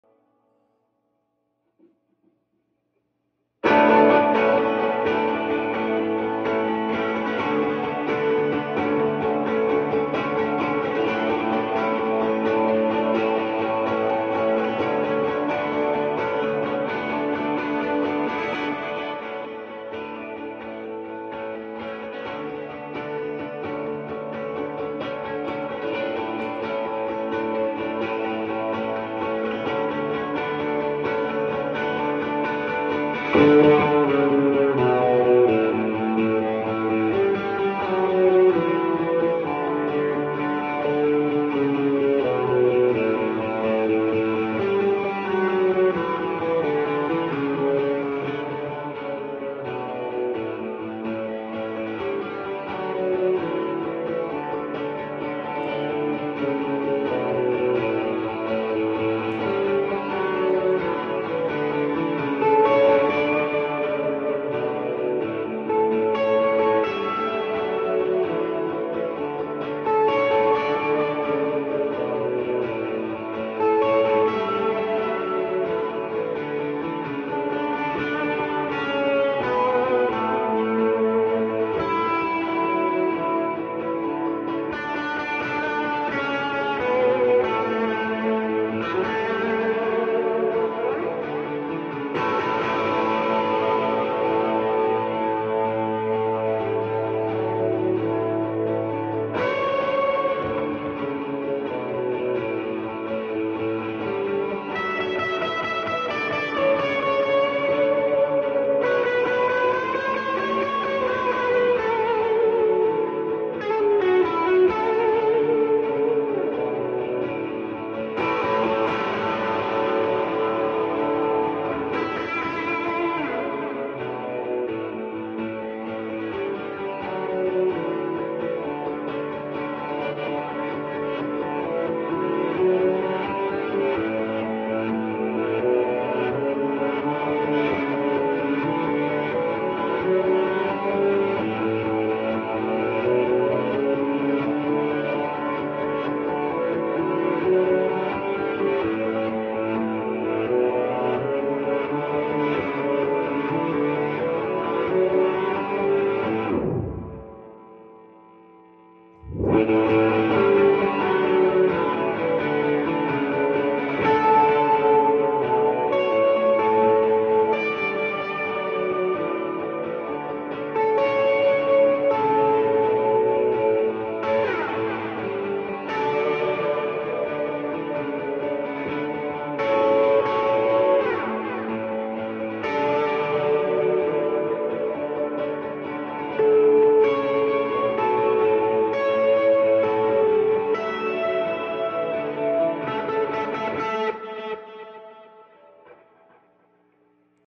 Collings and Carrs
Using the Volante in stereo and utilizing its sound on sound looper is extremely fun! These amps pair extremely well! The Super Bee has the Carr Valiant 12 inch speaker and the Original Mercury has a Scumback J75 65 watt speaker. The mercury is also equipped with a genalex gold lion KT66. Please see below for a break down of what pedals were used:

First rhythm part:
Collings 290 -Origin Effects Cali76CD -Snouse BlackBox 2 Stage Pro Mod -Diamond Vibrato -Strymon Volante (both amps)-Strymon Flint (Just the Original Mercury)

Phasery walk down:

Collings 290 - Origin Effects Cali76CD - Whirlwind Orange Box Phaser - Nordland ODR-C 30th anniversary -Diamond Vibrato -Strymon Volante (both amps)-Strymon Flint (Just the Original Mercury)

Other lead lines:

Collings 290 -Origin Effects Cali76CD -Greer Lightspeed -Diamond Vibrato -Strymon Volante (both amps)-Strymon Flint (Just the Original Mercury)